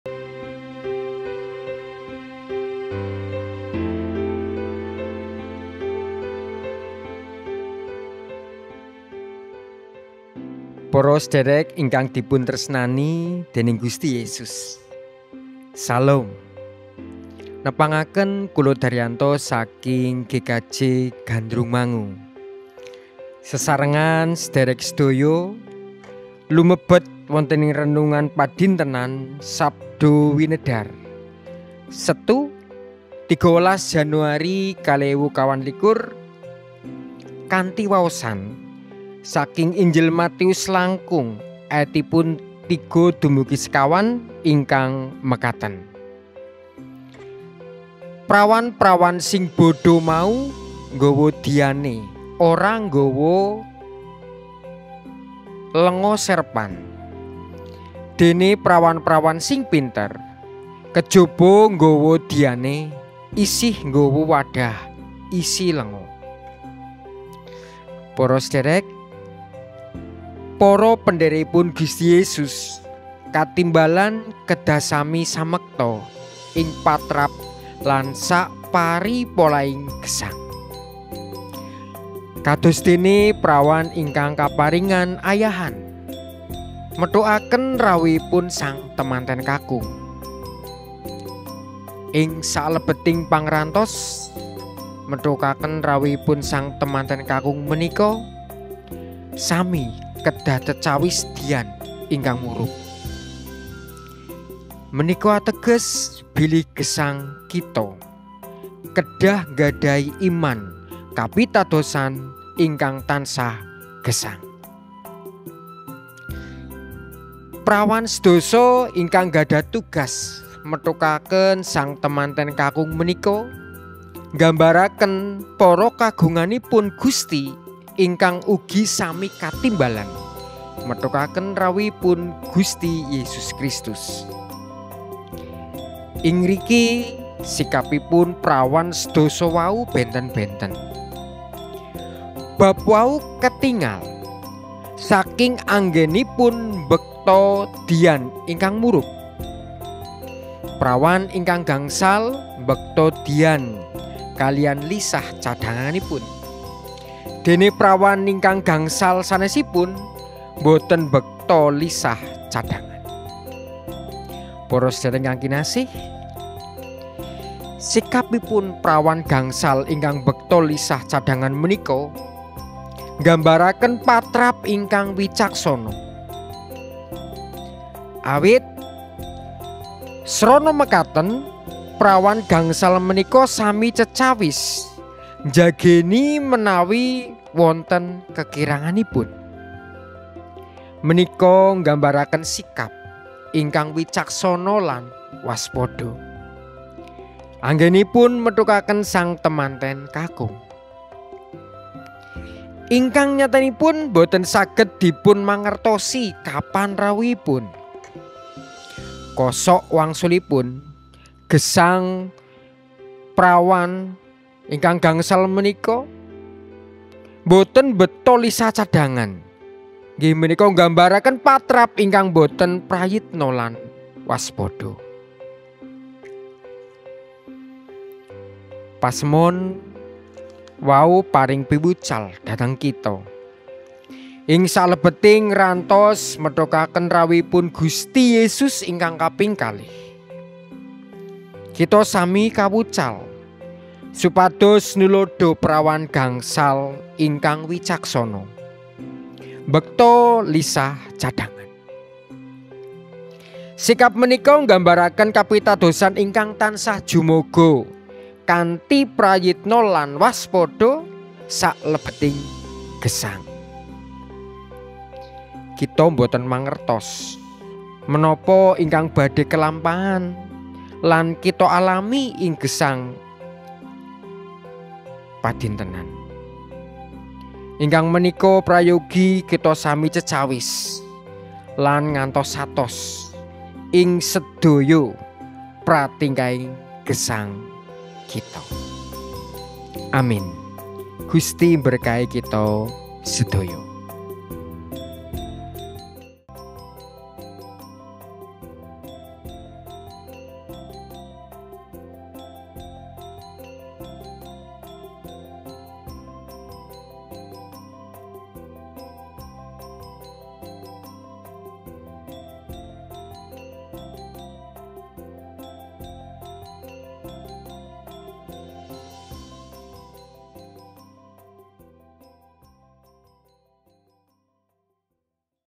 Sabda Winedar Edisi #013 Tanggal 13-Januari-2024
"Renungan Harian Sadhar - Sabda Winedar Edisi  #013 Tanggal 13-Januari-2024
ALLAH KANG MAHAMIRSA
“Dhuh Yehuwah, Paduka ingkang njajagi lan nguningani kawula.
Kawula linggiha utawi ngadega Paduka uninga, Paduka mirsa osiking
manah kawula saking katebihan” (Jabur 139:1-2)
 dibawakan oleh : Bpk. Daryanto
Kini Renungan Sadhar tersedia untuk smartphone, unduh sekarang juga Renungan Sadhar di playstore"